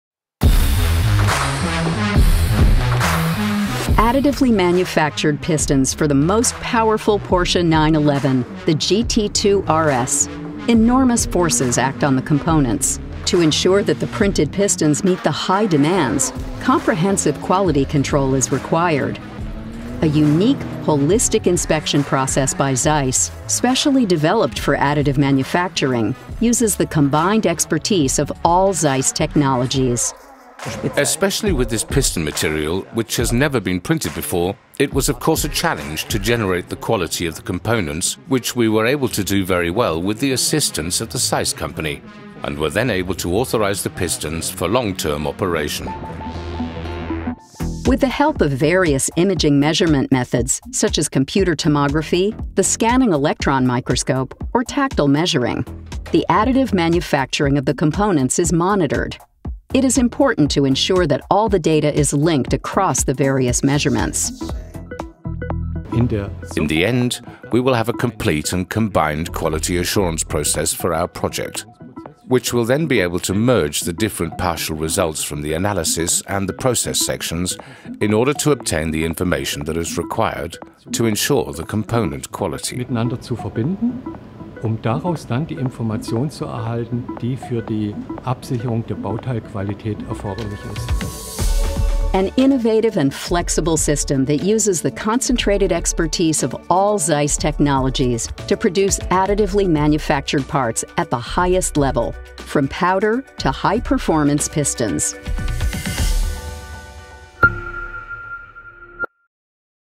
Quality assured AM manufacturing process
Quality assurance is essential across the many individual steps of the manufacturing process, from powder to finished component. To meet the specific requirements of this kind of manufacturing, a comprehensive quality assurance process has been developed by ZEISS. The goal of the project, apart from manufacturing the Porsche 911 piston prototypes and thus improving the efficiency of the components, was to develop an additive manufacturing process capable of meeting the highest quality standards while remaining lean and cost-efficient to make series production possible.